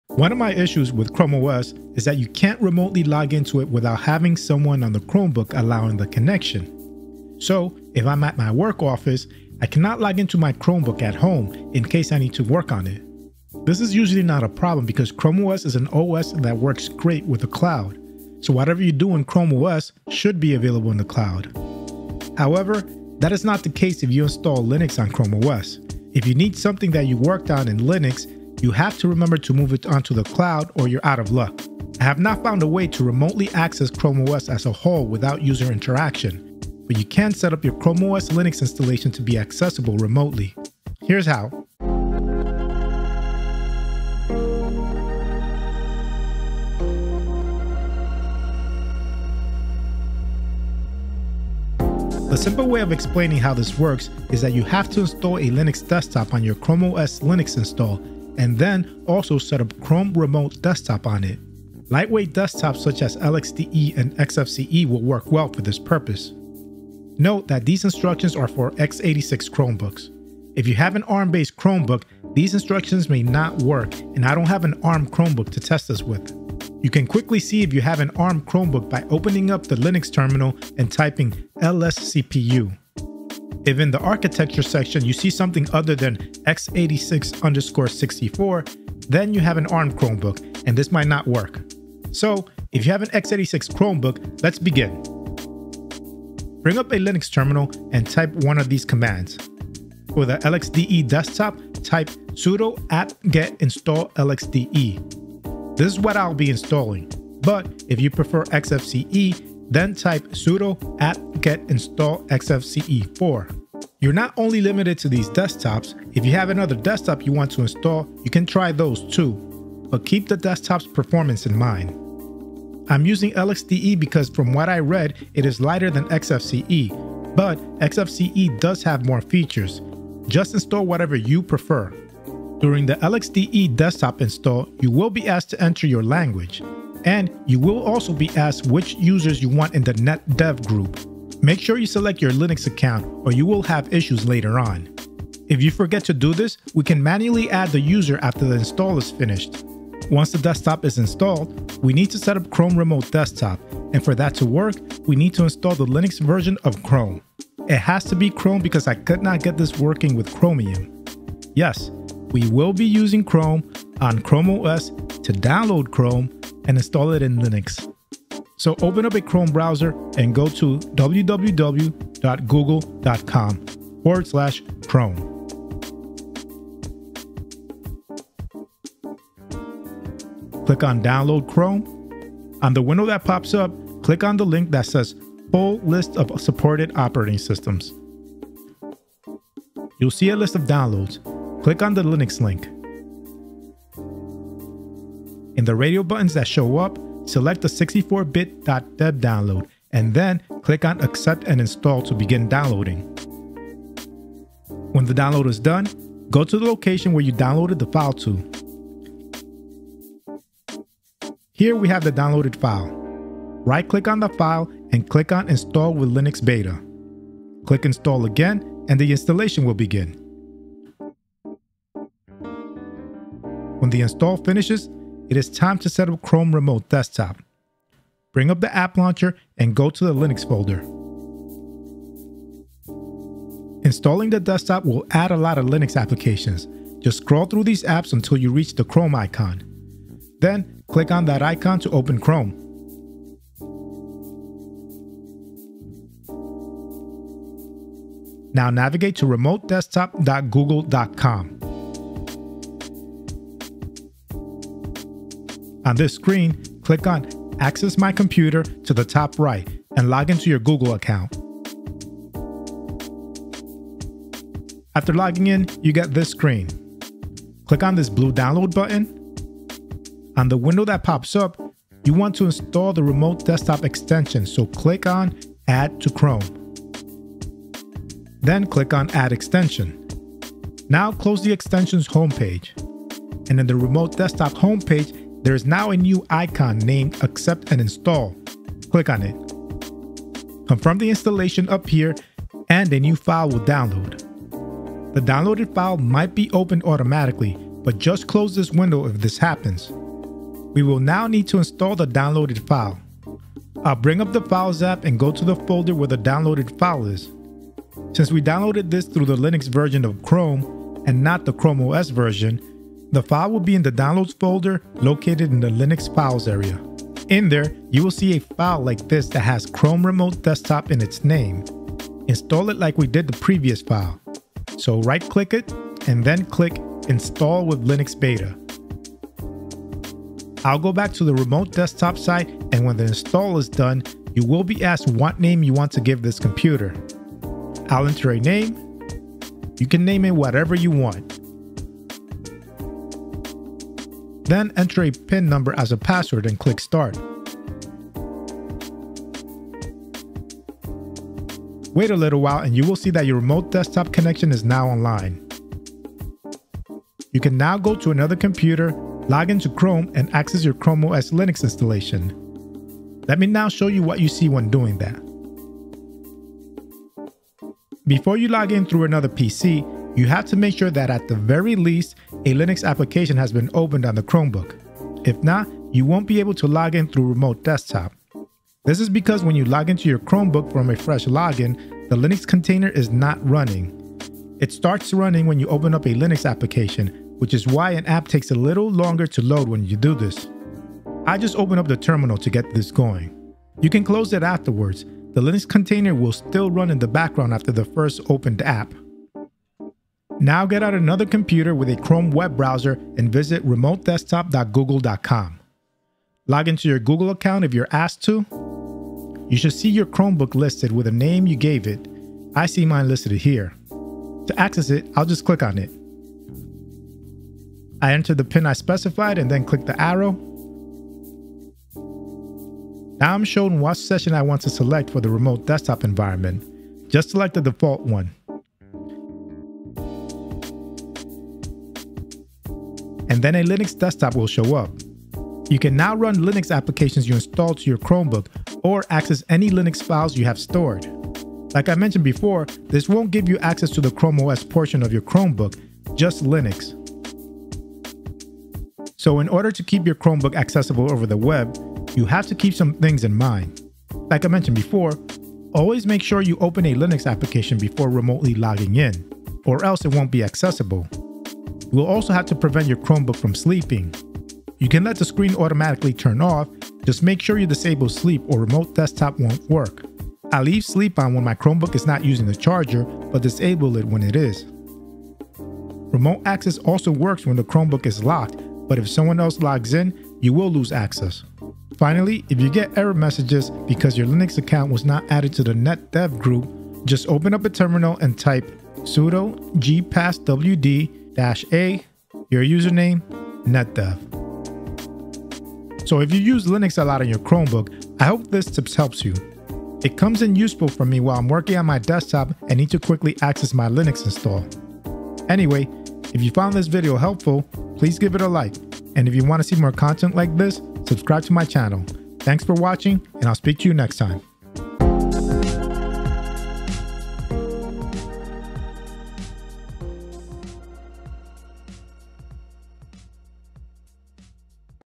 Unattended Remote Desktop in Chrome OS Linux
Remoting into a Chromebook is currently only possible by someone on a Chromebook giving permissions to a remote user. However, it is possible to get unattended access to the Linux portion of Chrome OS.

This is accomplished by installing a Linux desktop, Chrome for Linux and then setting up Chrome Remote Desktop on the Linux portion of Chrome OS. Check out the video to see the entire process.

Helpful Linux commands:
lscpu - Lets you check what CPU type you have.
sudo apt-get install lxde - Install a LXDE desktop for use when you remote access into Chrome OS
sudo apt-get install xfce4 - Install a XFCE desktop for use when you remote access into Chrome OS
sudo gpasswd -a YourUserName netdev - In case you get netdev errors after installing lxde

Time stamps
0:00 Intro
0:50 Quick explanation of how to do this
1:06 Do you have an ARM or x86 Chromebook?
1:34 Installing the Linux desktop
2:31 Installing the Linux version of Chrome OS
3:41 Setting up Chrome Remote Desktop
6:06 Before remoting into your chromebook do this
6:39 Logging into your Linux install with Remote Desktop
7:26 Things to know
8:14 Closing thoughts